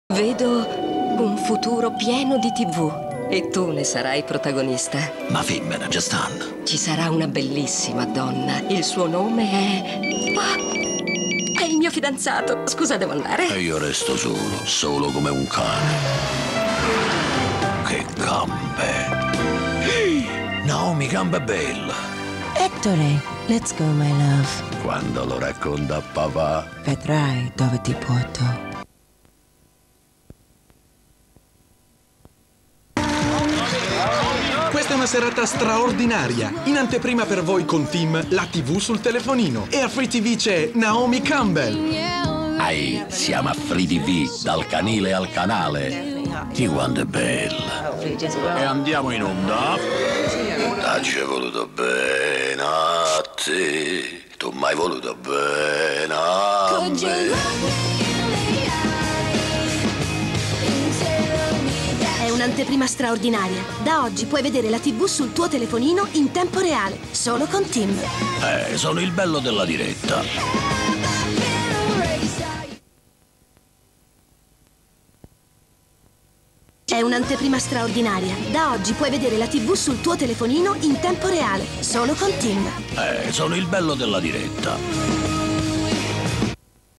Tim ETTORE Naomi Campbell
Serie di pubblicità TIM con il cane Ettore e Naomi Campbell.